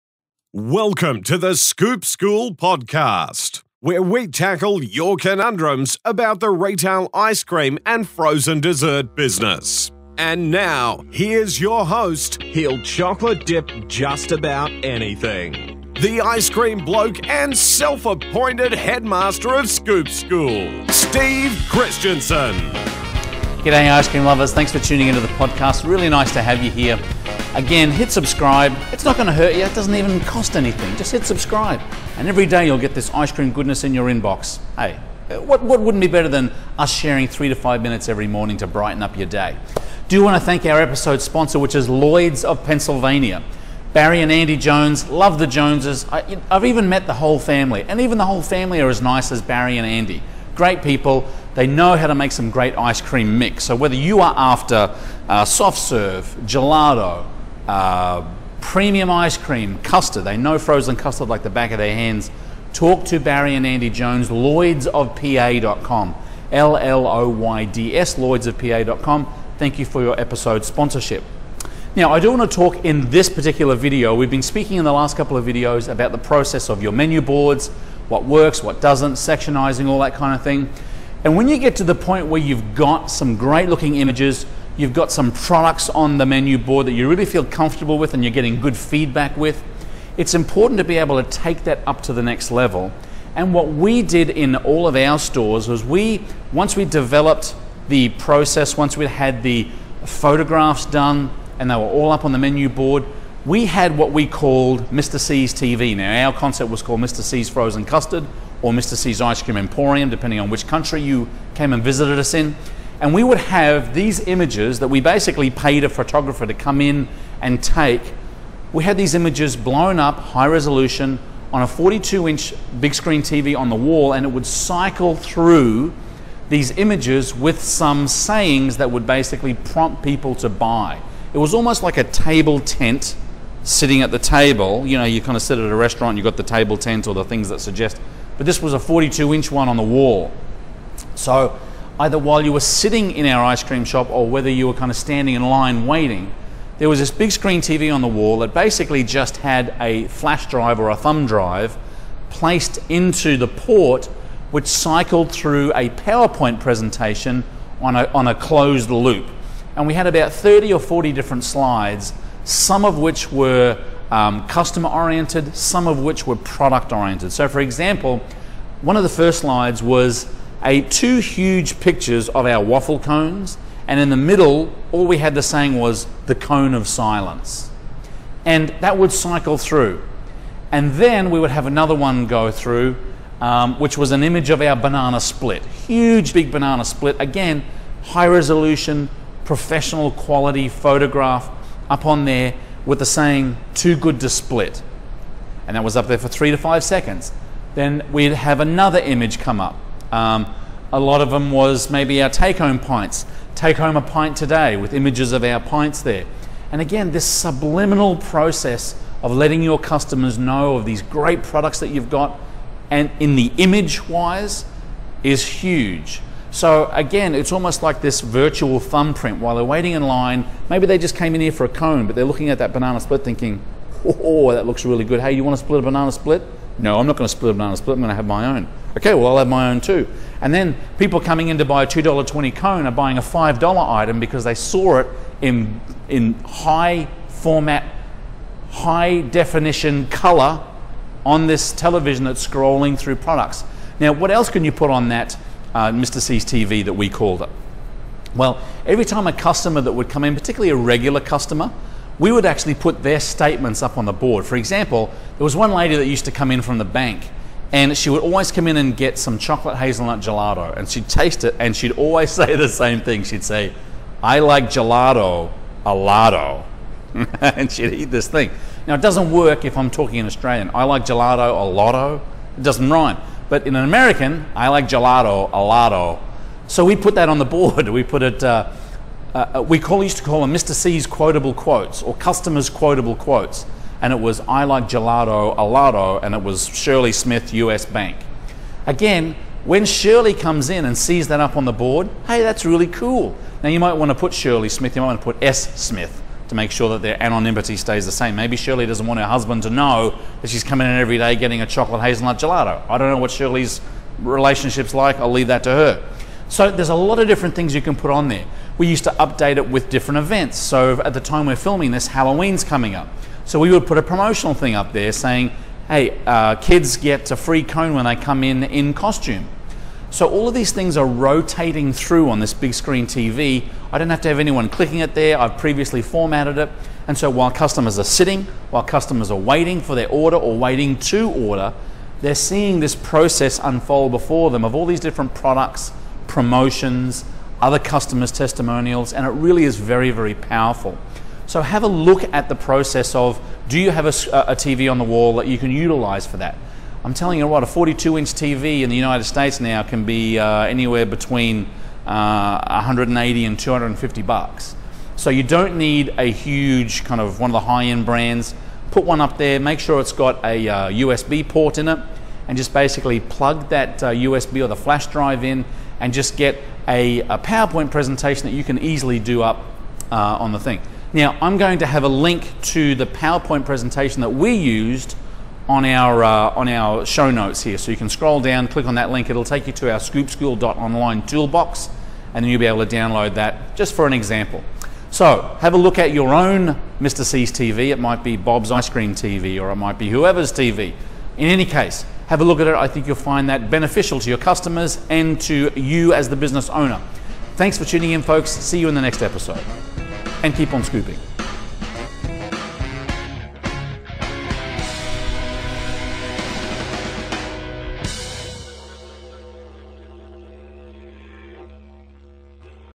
How to Promote your Menu Photos
How do you promote your menu images.

Steve Christensen, The Ice Cream Bloke and Self-Appointed Headmaster of Scoop School, talks in this episode about promoting your menu images

Download a copy of our powerpoint slideshow Register for free and find the presentation in the Toolbox

For more information on our Scoop School programs or how to open or grow your own ice cream or frozen dessert business, go